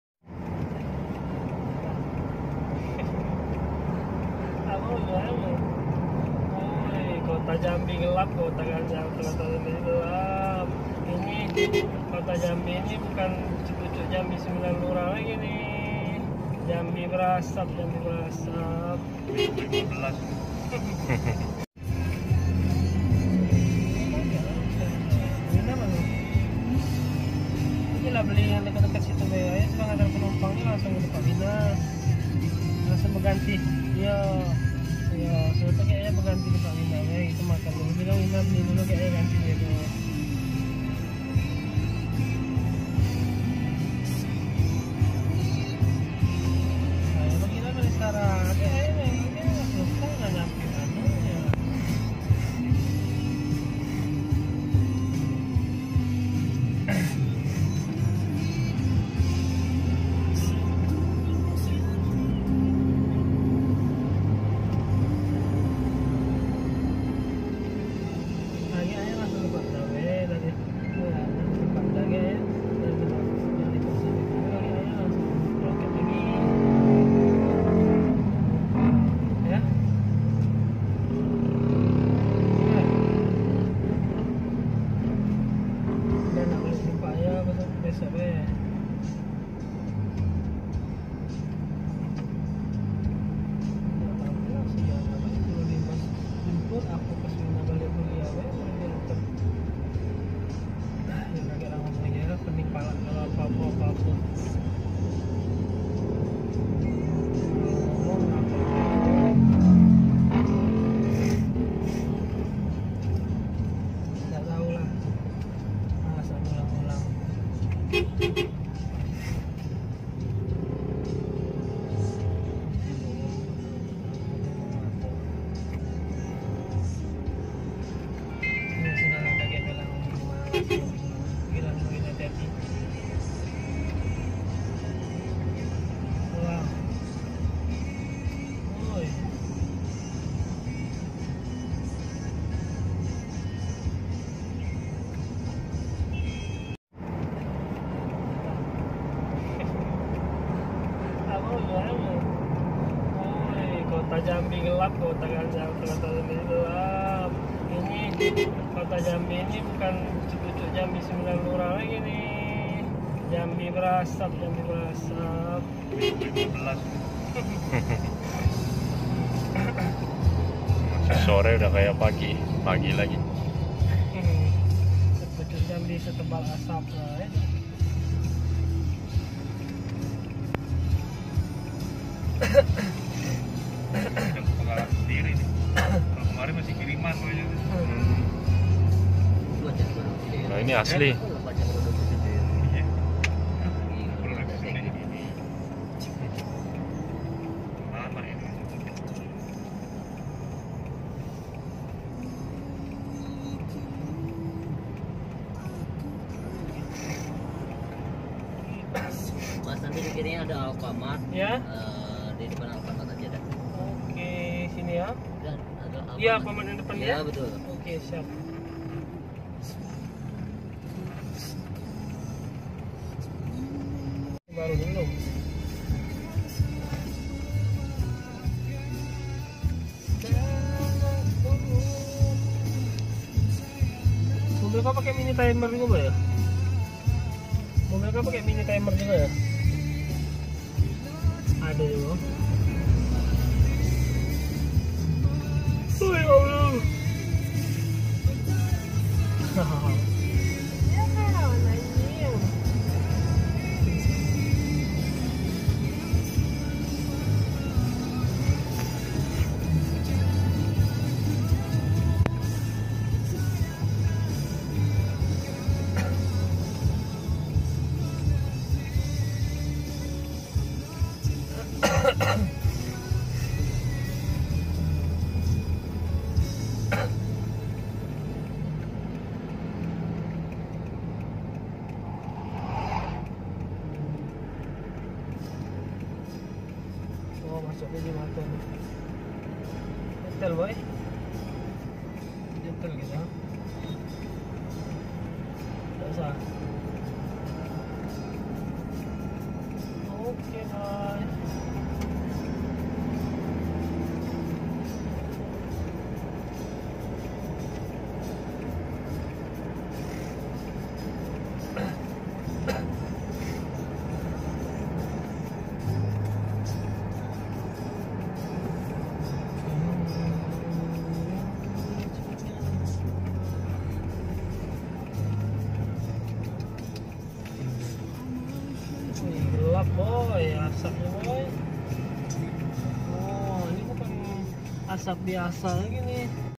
Kabut Asap Terparah 2019 Kota Jambi, Jarak Pandang Hanya 1 Meter
Kabut asap akibat kemarau panjang dan kebakaran hutan yang melanda Kota Jambi di tahun 2019